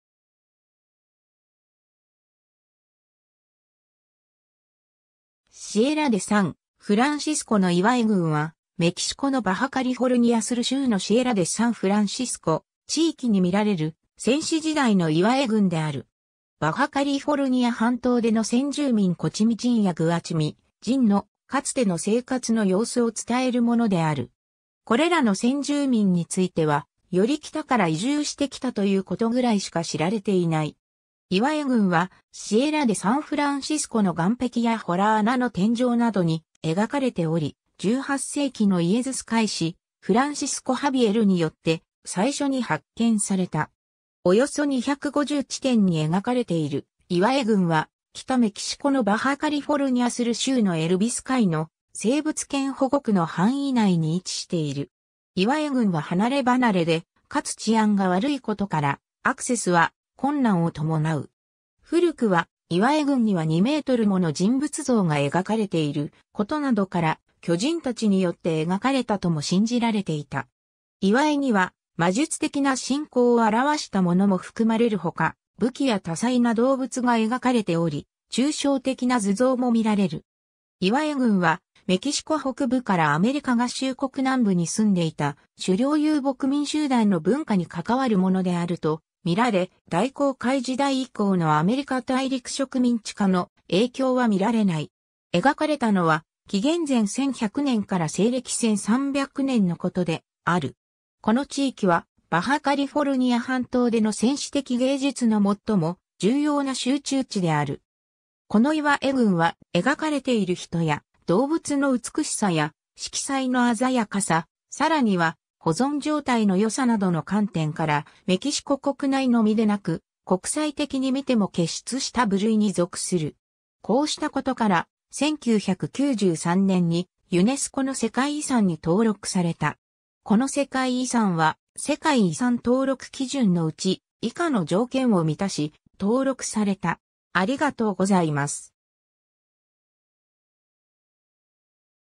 シエラ・デ・サン・フランシスコの岩絵群
バハ・カリフォルニア・スル州
シエラ・デ・サン・フランシスコの岩絵群は、メキシコのバハ・カリフォルニア・スル州のシエラ・デ・サン・フランシスコ（サンフランシスコ山地）地域に見られる先史時代の岩絵（ペトログリフ）群である。
 バハ・カリフォルニア半島での先住民コチミ人（Cochimi）やグアチミ人（Guachimis）のかつての生活の様子を伝えるものである。
これらの先住民については、より北から移住してきたということぐらいしか知られていない。
 岩絵群は、シエラ・デ・サンフランシスコの岩壁や洞穴の天井などに描かれており、18世紀のイエズス会士フランシスコ・ハビエルによって最初に発見された。
およそ250地点に描かれている岩絵群は、北メキシコのバハ・カリフォルニア・スル州のエル・ビスカイノ生物圏保護区の範囲内に位置している。
岩絵群は離れ離れで、かつ治安が悪いことから、アクセスは困難を伴う。
 古くは、岩絵群には2mもの人物像が描かれていることなどから、巨人たちによって描かれたとも信じられていた。
岩絵には魔術的な信仰を表したものも含まれるほか、武器や多彩な動物（ウサギ、ピューマ、ヤマネコ、シカ、ヤギ、クジラ、カメ、マグロ、イワシ、タコ、ワシ、ペリカンなど）が描かれており、抽象的な図像も見られる。
 岩絵群はメキシコ北部からアメリカ合衆国南部に住んでいた狩猟遊牧民集団の文化に関わるものであると見られ、大航海時代以降のアメリカ大陸植民地化の影響は見られない。
描かれたのは紀元前1100年から西暦1300年のことである。
 この地域は、バハ・カリフォルニア半島での先史的芸術の最も重要な集中地である。
この岩絵群は、描かれている人や動物の美しさや色彩の鮮やかさ、さらには保存状態の良さなどの観点から、メキシコ国内のみでなく国際的に見ても傑出した部類に属する。
こうしたことから、1993年に、ユネスコの世界遺産に登録された。